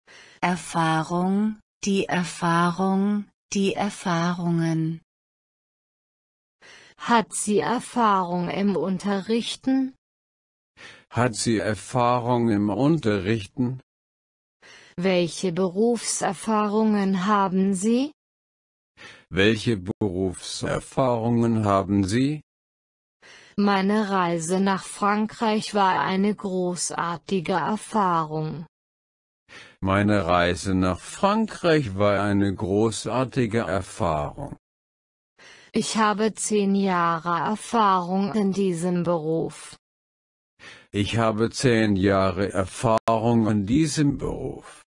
Wortschatz A2: Erfahrung
A2 Vocabulary: die Erfahrung, die Erfahrungen
Hat sie Erfahrung im Unterrichten?
Welche Berufserfahrungen haben Sie?
Meine Reise nach Frankreich war eine großartige Erfahrung.
Ich habe zehn Jahre Erfahrung in diesem Beruf.